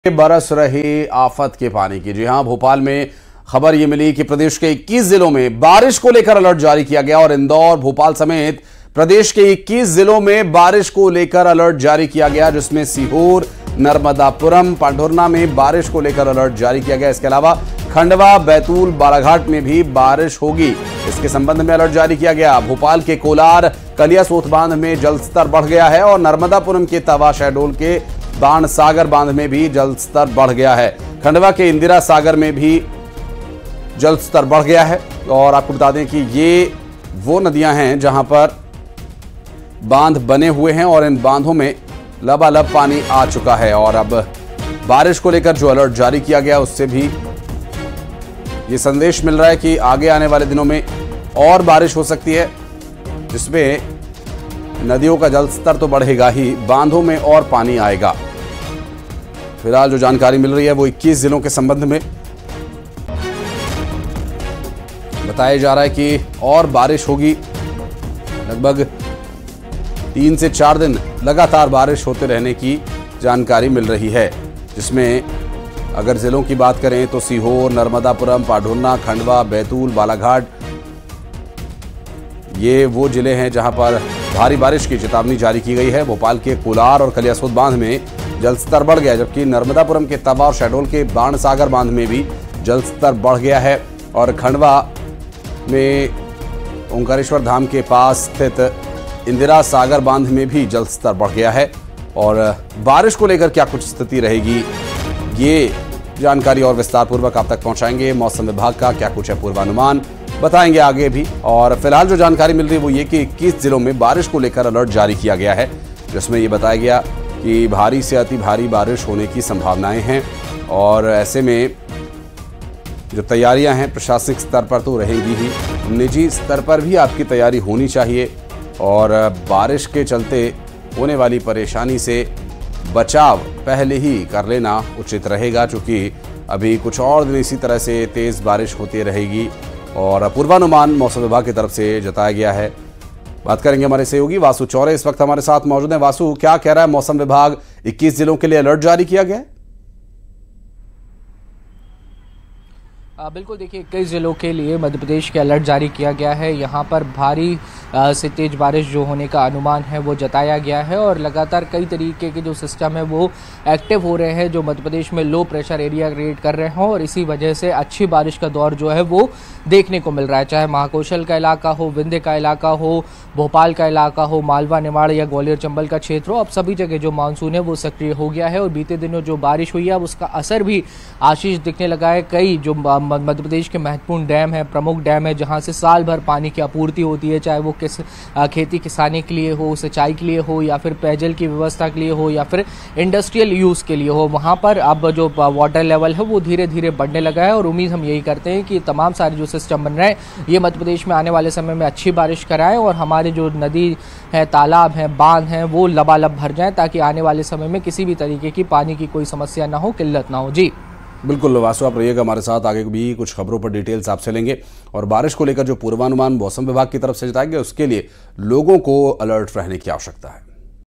MP Weather Update : बारिश ने मौसम किया खुशनुमा, MP में बरसात का अलर्ट जारी | Bhopal Rain | MP News
एमपी में मौसम का मिजाज बदल गया है. मौसम विभान ने राजधानी भोपाल समेत 21 जिलों में भारी बारिश का अलर्ट जारी किया गया है. जानिए कौन-कौन से जिलों में बारिश के अलर्ट जारी है...